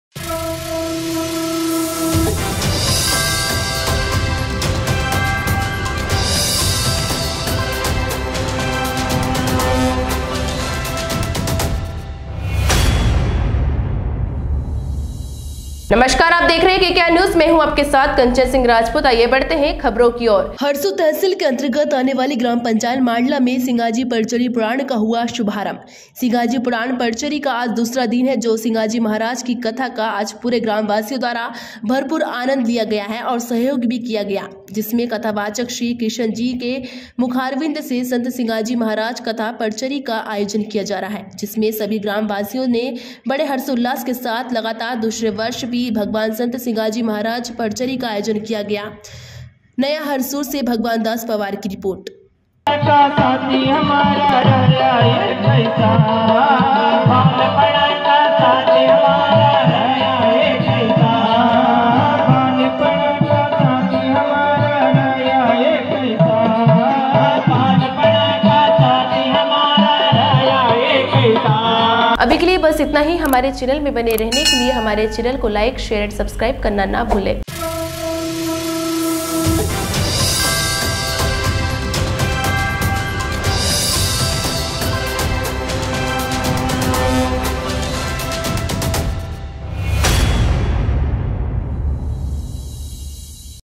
हरसूद तहसील केअंतर्गत आने वाली ग्राम पंचायत मांडला। में सिंगाजी परचरी पुराण का हुआ शुभारंभ।
नया हरसूद                                 हरसूद तहसील केअंतर्गत आने वाली ग्राम पंचायत मांडला। में सिंगाजी परचरी पुराण का हुआ शुभारंभ।       सिंगाजी पुराण परचरी का आज दूसरा दिन जो सिंगाजी महाराज की कथा का आज पूरे ग्राम वासियों द्वारा भरपूर आनंद लिया गया और सहयोग भी किया गया जिसमें कथावाचक श्री किशन जी के मुखारविंद से संत सिंगाजी महाराज कथा परचरी का आयोजन किया जा रहा है जिसमें सभी ग्राम वासियों ने बड़े हर्षोल्लास के साथ  लगातार दूसरे वर्ष भी भगवान संत सिंगाजी महाराज परचरी का आयोजन किया गया।                                नया हरसूद से भगवानदास पवार की रिपोर्ट